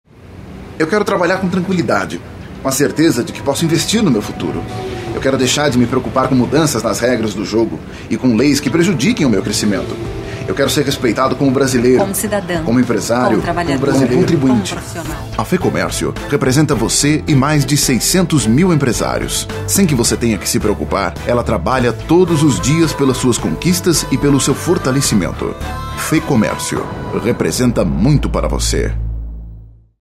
FECOMERCIO.mov
"Monstro" do comercial FECOMERCIO " Eu e mais 600mil" Conceito criativo e soluções audiovisuais da SETE18.